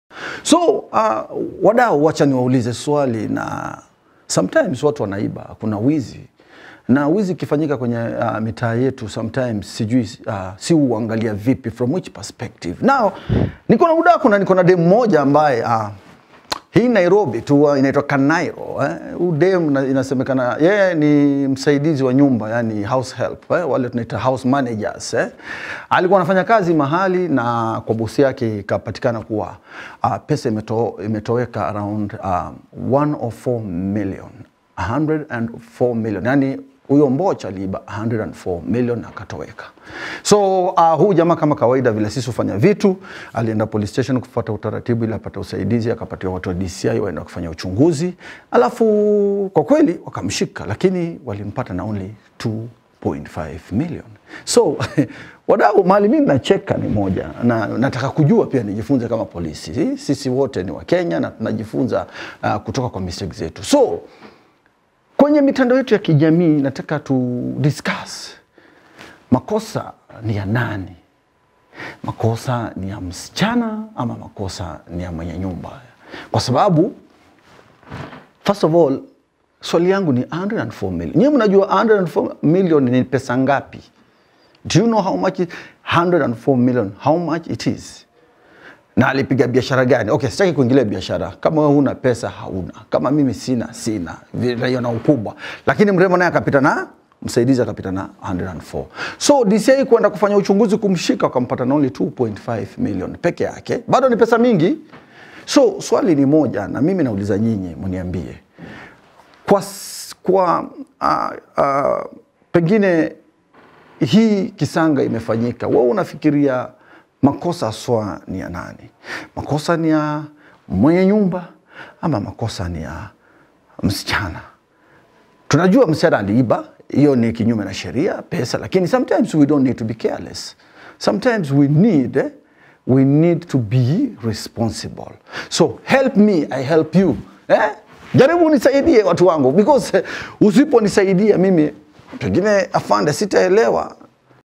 "Who Is To Blame For The Loss Of Ksh 104 Million Stolen Recently By A House Help?" Qwachezz Asks
Administered by Ngomma Value Added Services